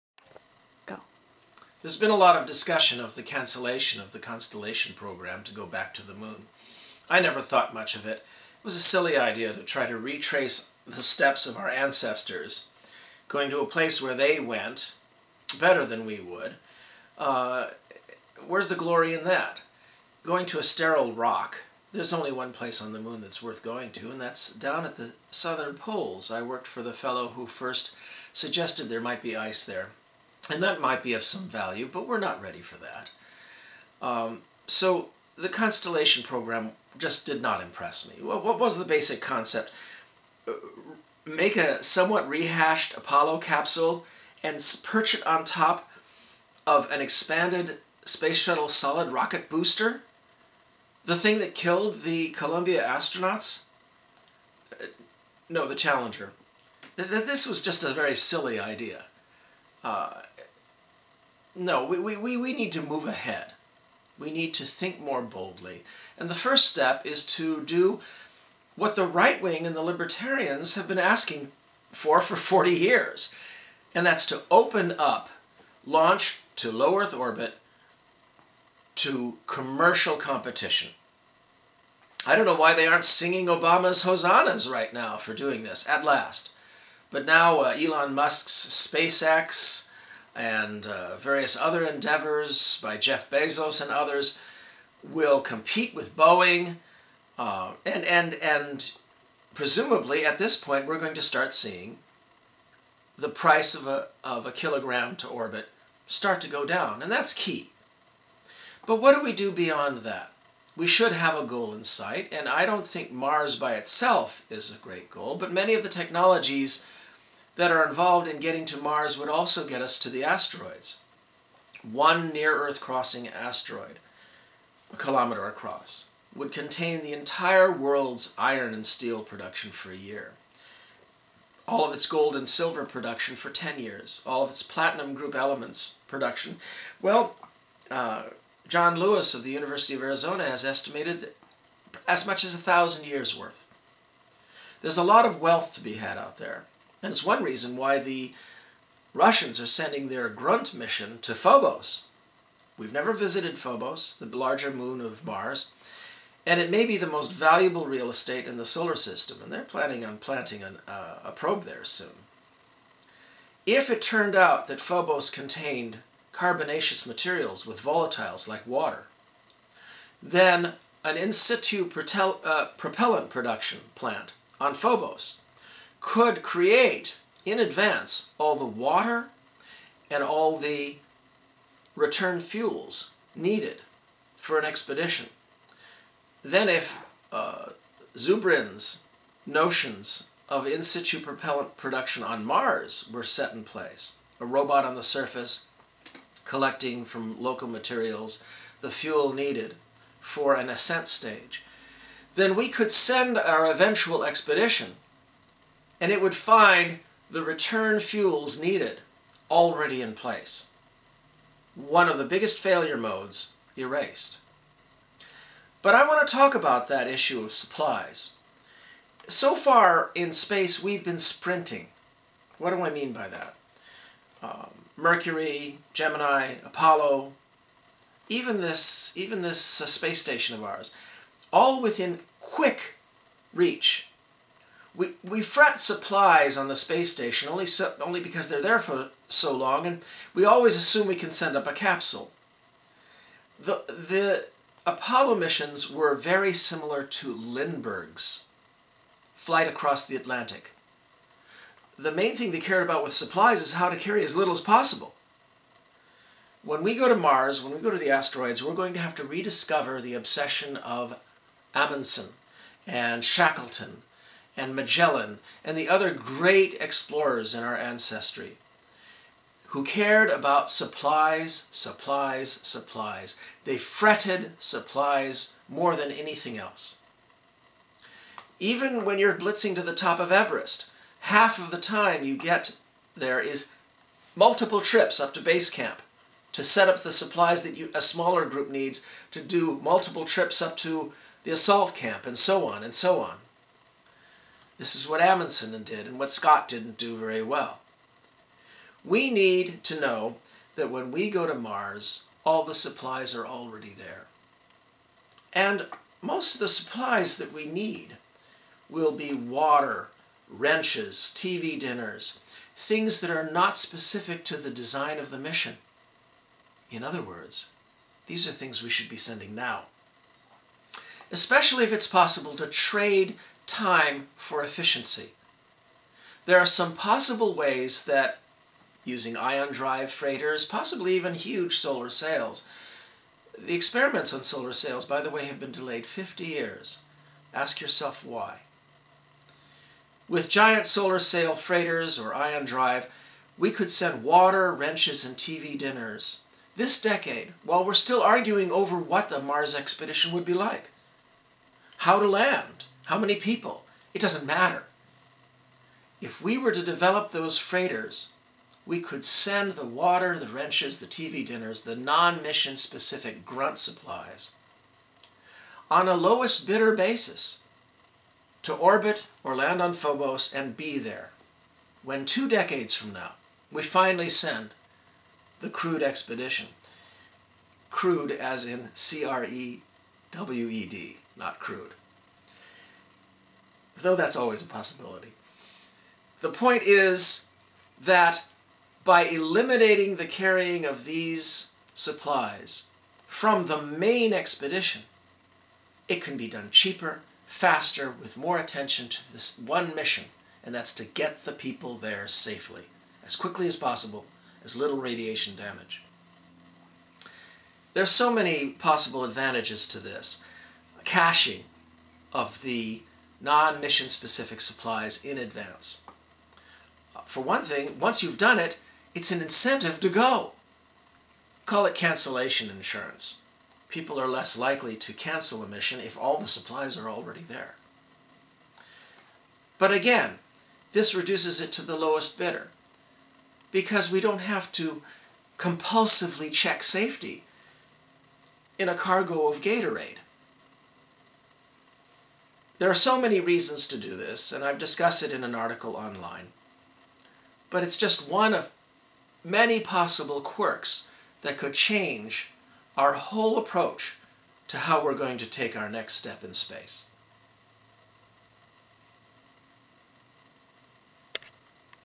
Space Exploration Part 1:Planning our next steps in space
Science fiction author David Brin speculates on the future of the space program. Where should we be going? Brin suggests caching supplies ahead of sending a manned mission. David Brin is the author of books such as The Postman, Startide Rising, The Transparent Society and The Uplift War.